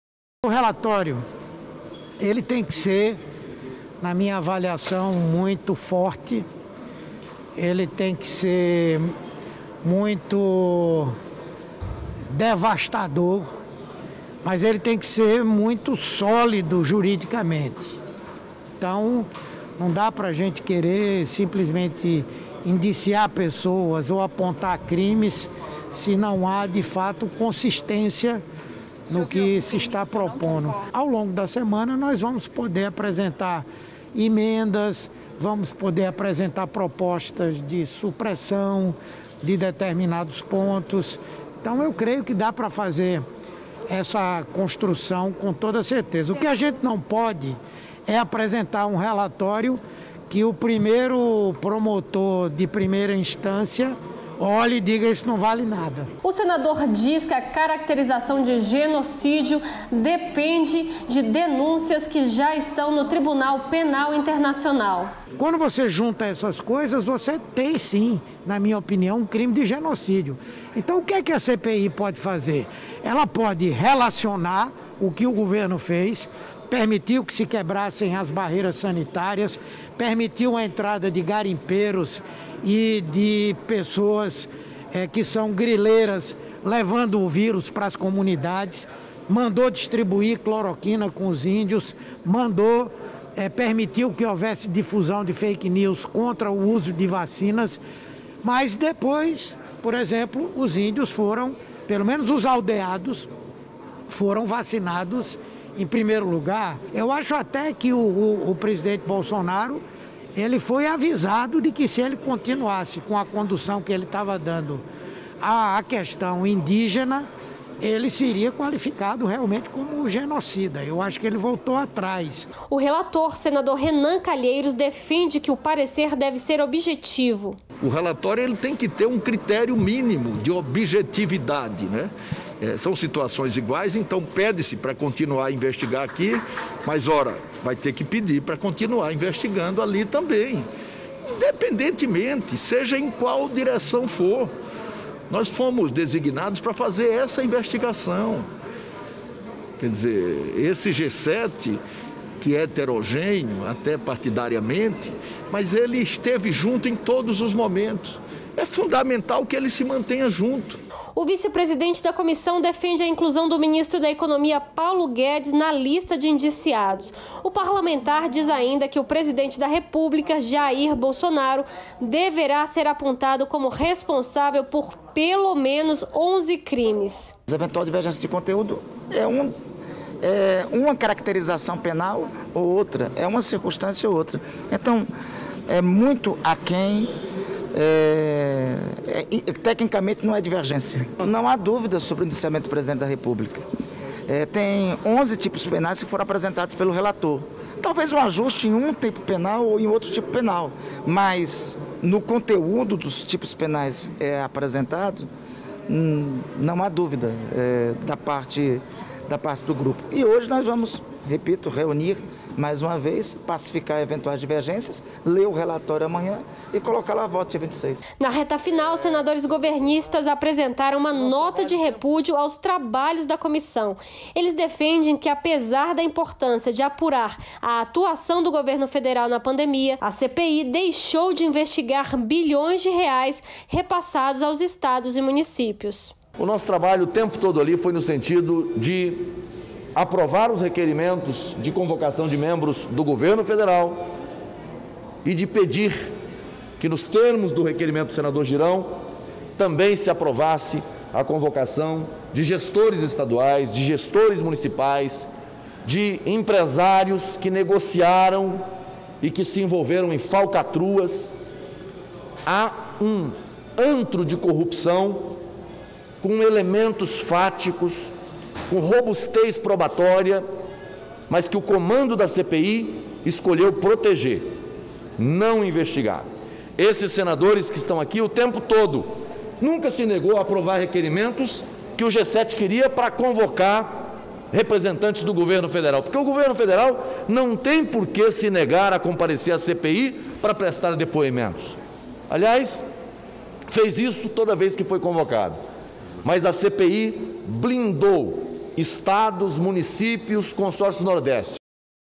Relatório da CPI da Pandemia será apresentado nesta quarta-feira com sugestões do G7
Senadores integrantes da CPI da Pandemia e que fazem parte do grupo majoritário, conhecido como G7, propuseram ao relator, senador Renan Calheiros (MDB-AL), alterações e adições ao texto que será apresentado nesta quarta-feira (20). A votação do parecer só deve acontecer na próxima terça-feira (26).